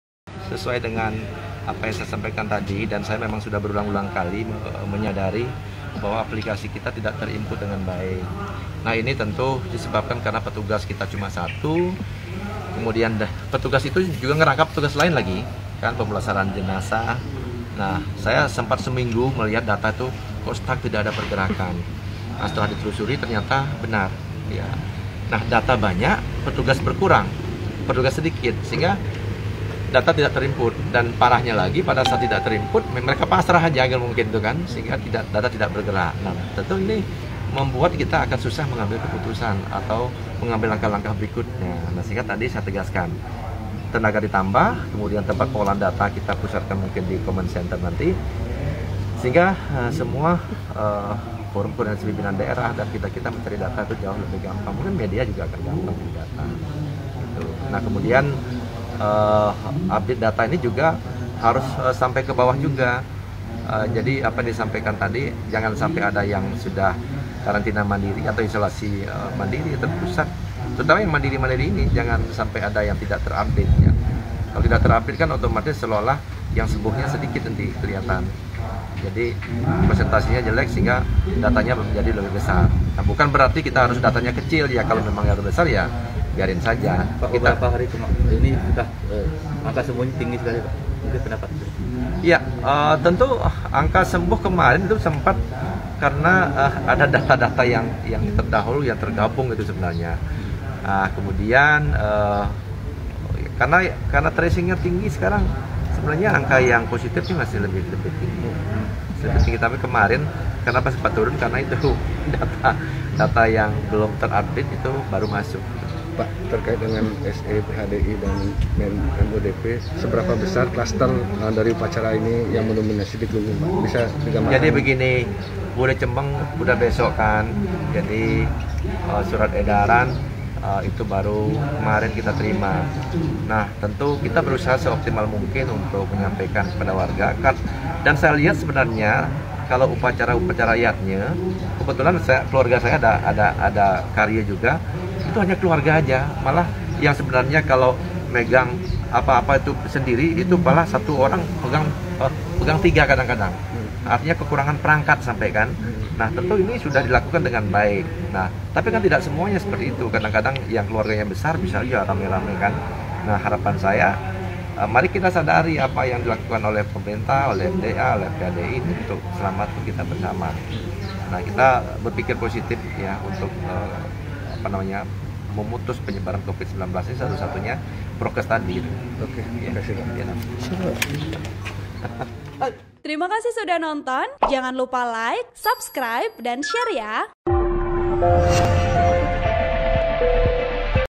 Penanggulangan Penyebaran Covid-19 di Klungkung
Penjelaskan Ketua Satgas I Nyoman Suwirta terkait penanggulangan penyebaran Covid-19 di Klungkung, Selasa (9/8/2021).

(IAG)